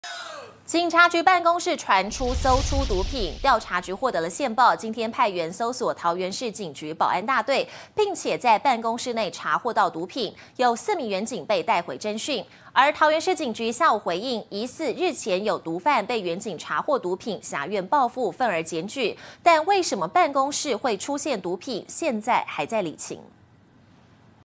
桃市保大辦公室搜出毒品 4警帶回偵訊
警察局辦公室，驚傳搜出毒品！調查局獲得線報，今天（１９）派員搜索桃園市警局保安大隊，並在辦公室內查獲毒品，4名員警被帶回偵訊。桃園市警局下午回應，疑似是日前有毒販被員警查獲毒品，挾怨報復憤而檢舉，但為何辦公室會出現毒品，目前案件仍在釐清中。